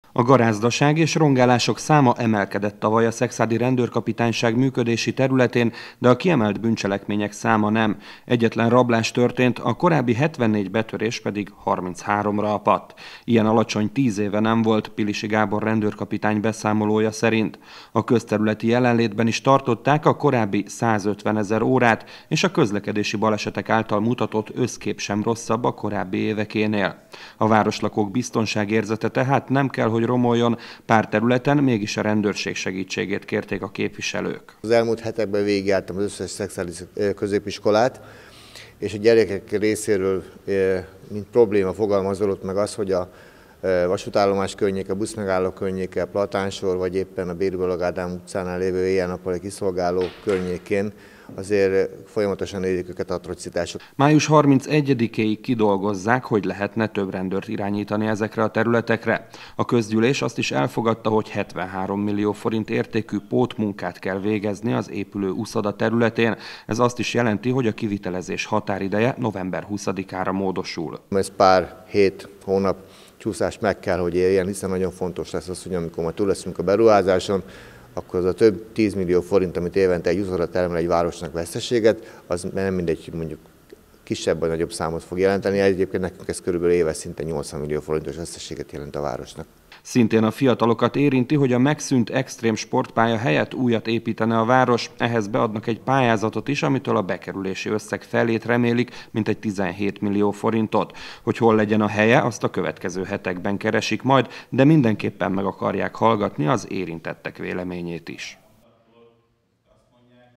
Tovább javítanák a közbiztonságot Szekszárdon - ülésezett a városi közgyűlés 20190425
Május 31-ig közös tervet dolgoz ki a rendőrség és a szekszárdi önkormányzat, hogy miként lehet tovább javítani a közbiztonságot a megyeszékhely egyes részein. A közgyűlés a rendőrkapitányság 2018-as beszámolóját elfogadta. Eszerint a kiemelt bűncselekmények száma nem emelkedett Szekszárdon és környékén.